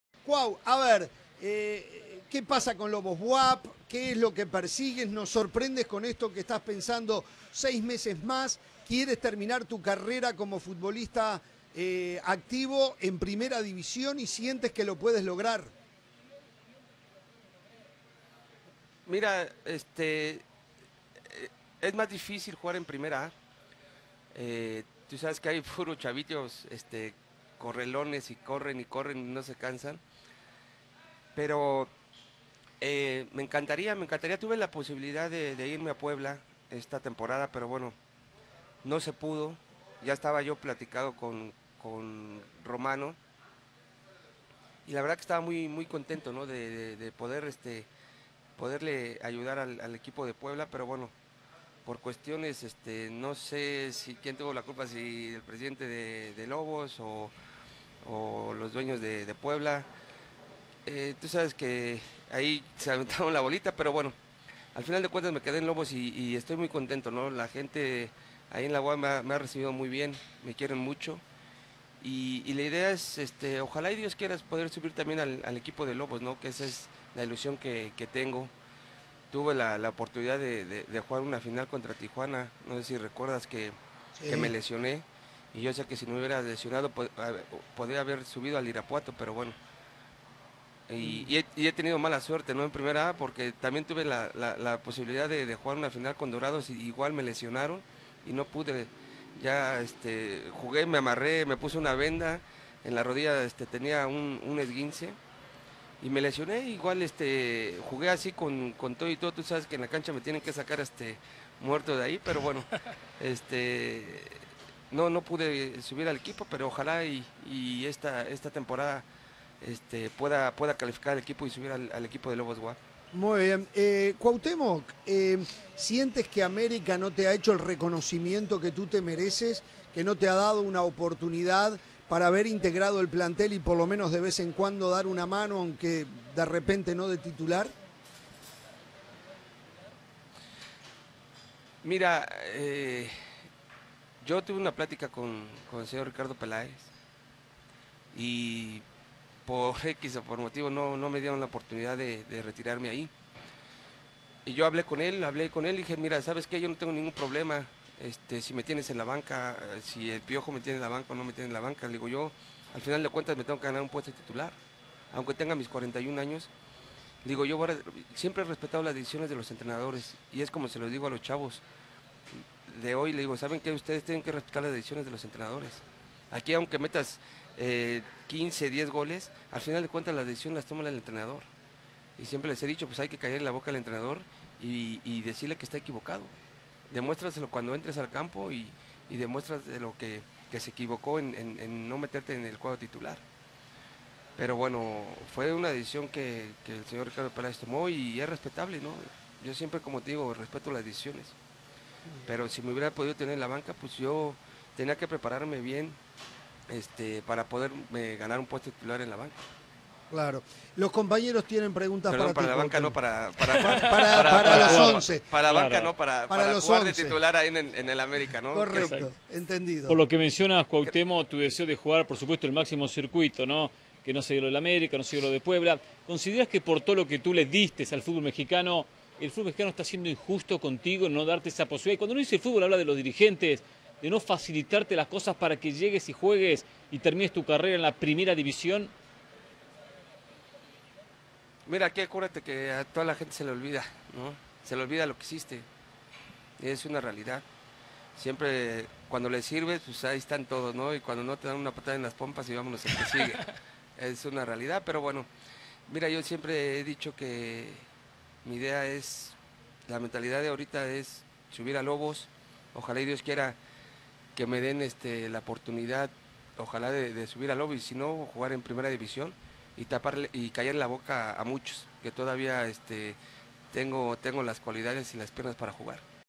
Cuauhtémoc Blanco no quiere retirarse
Cuauhtémoc Blanco dijo que si logra ascender a la Primera División con Lobos BUAP, contempla la posibilidad de jugar una temporada más.